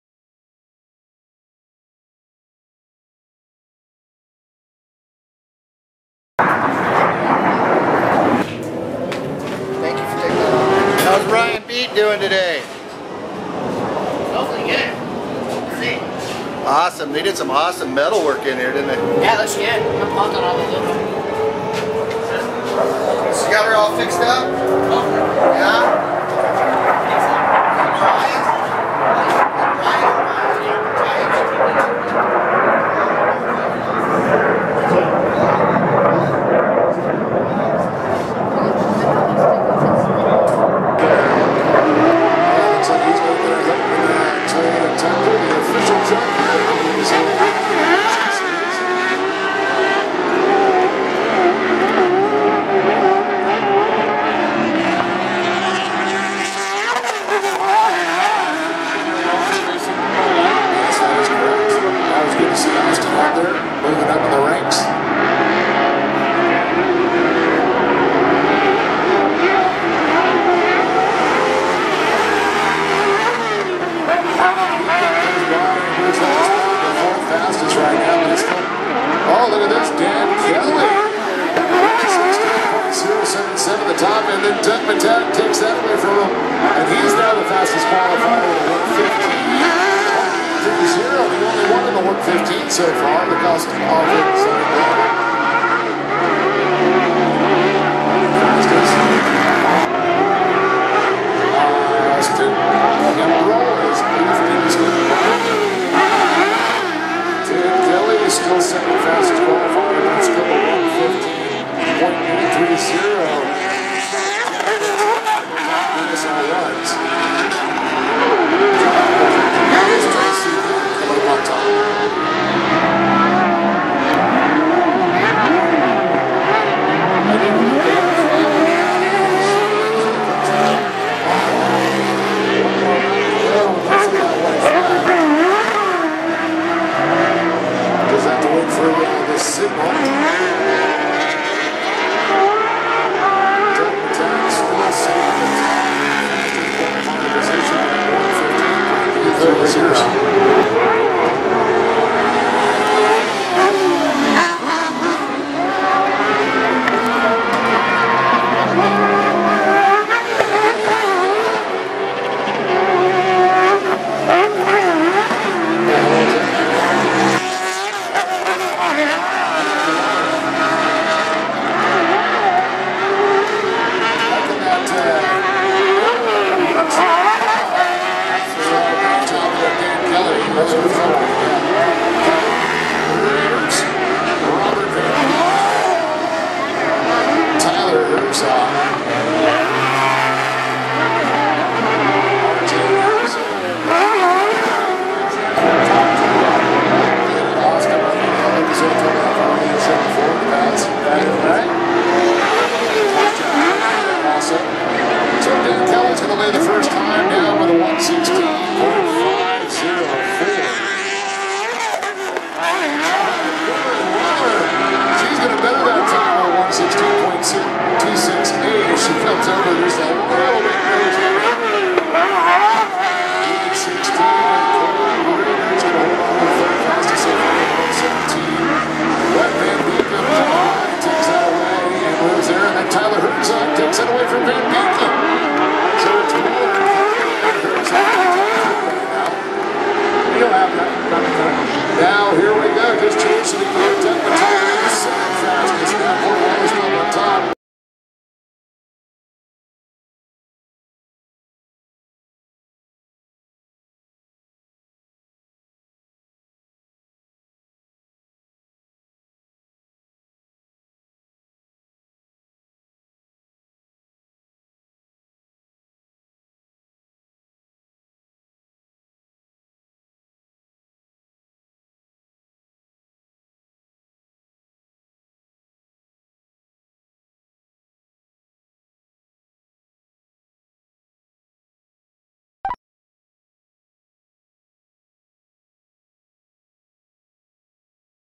Ryan Beat fast qualifier 11-5-10 LVMS
Ryan Beat #851 SR1 Rhino Friday at Las Vegas Motor Speedway for the Lucas Oil Off Road Racing Series short course fast qualifier.